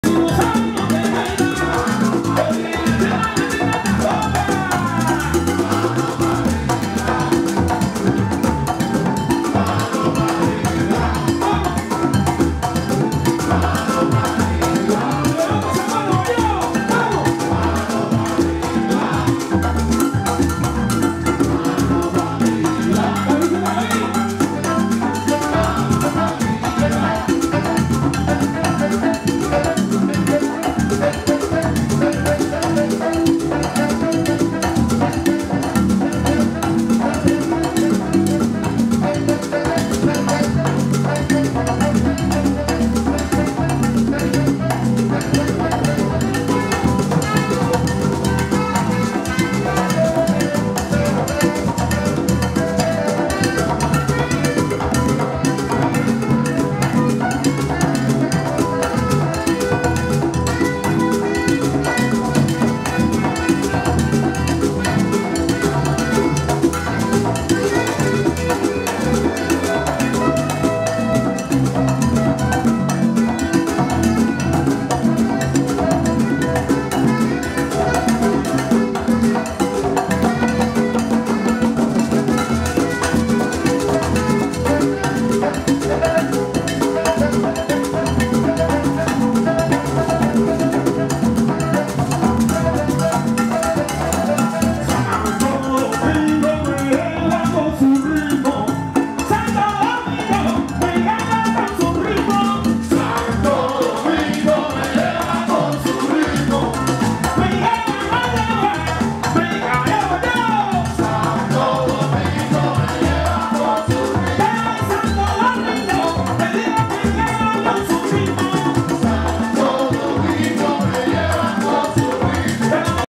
El Dominican Republic Jazz Festival en Cabarete, así se improvisa
La fusión de ritmos entre músicos internacionales improvisando jazz en la presentación de Cabarete, nov. 2015.